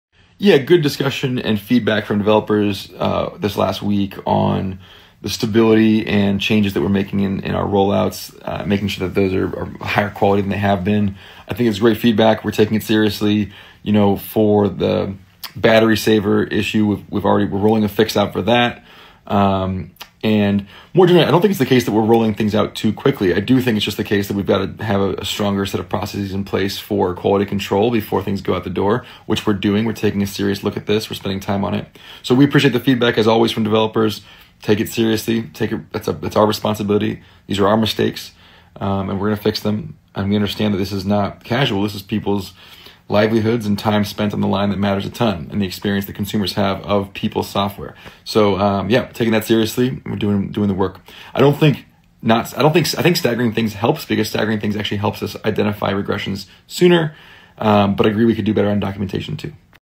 Meta CTO Responds To Quest Performance Regression Issues
Meta CTO Andrew Bosworth has responded to developer concerns over Quest's Horizon OS performance regression and the puzzling Battery Saver change.